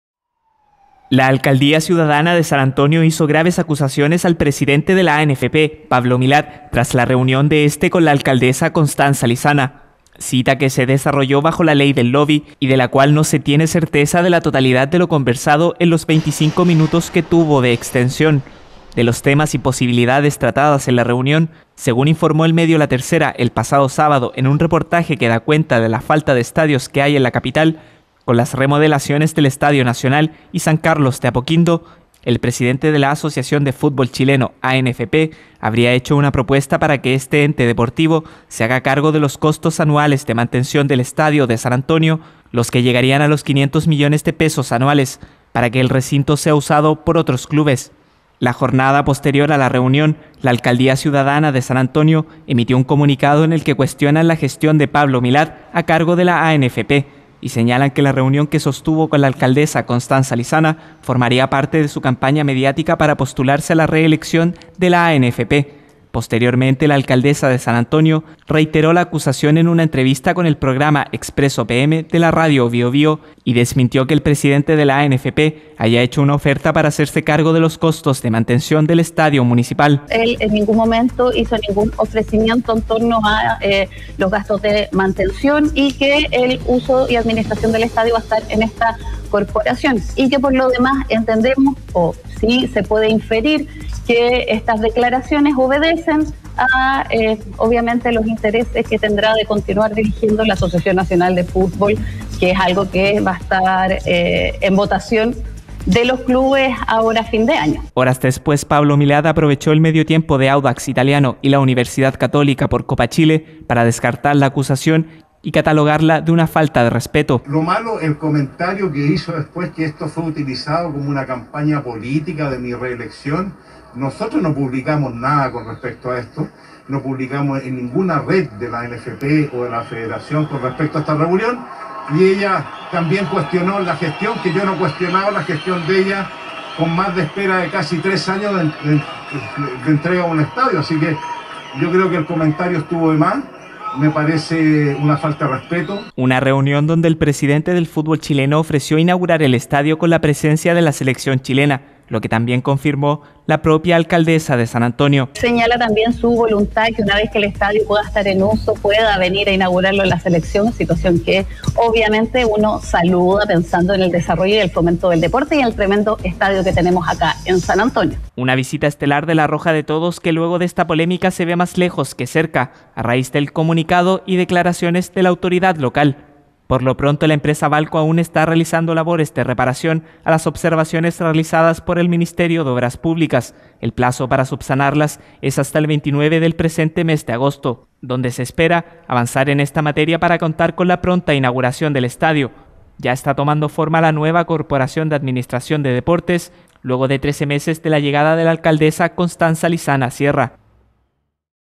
17 AGO 2022 Municipio de San Antonio acusa al presidente de la ANFP de hacer campaña mediática
Siguen las polémicas en torno al nuevo Estadio Municipal Doctor Olegario Henríquez. Todo esto tras las acusaciones de la Alcaldía ciudadana de San Antonio al presidente de la Asociación de Fútbol Chileno, Pablo Milad, a raíz de una reunión que tuvo la autoridad de la ANFP con la alcaldesa Constanza Lizana. Informaciones sobre esta reunión y la polémica en la siguiente nota.